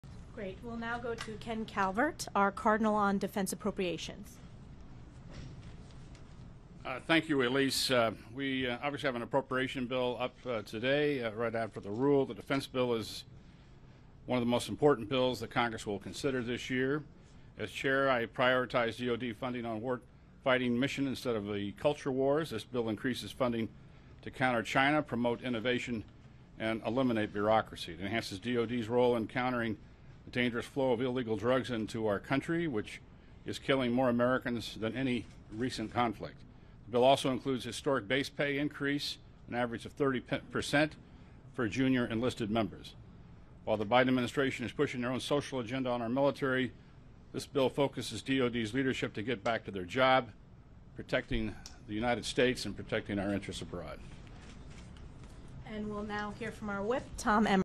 Ken Calvert Advocates For DoD Funding To Complete Missions ‘Instead Of The Culture Wars’
During a press briefing on Wednesday, Rep. Ken Calvert (R-CA) spoke about the defense bill.